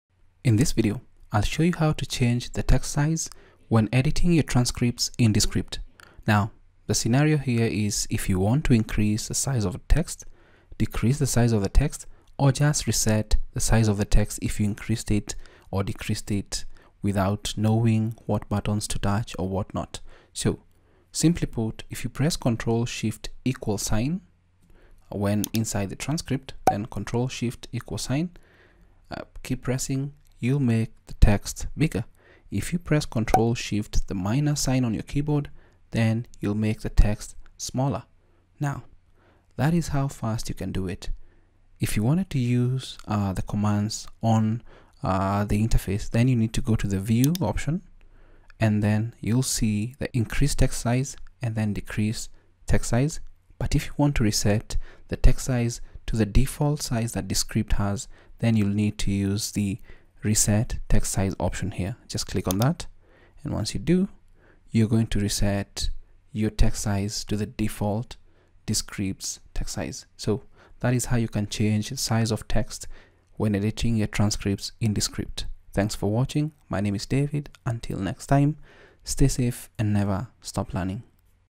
How to Change Text Size when Editing Transcripts in Descript Increase, Decrease and Reset Text Size
In this video, I'll show you how to Change Text Size when Editing Transcripts in Descript.Try Descript Now

I hope this video has been of value.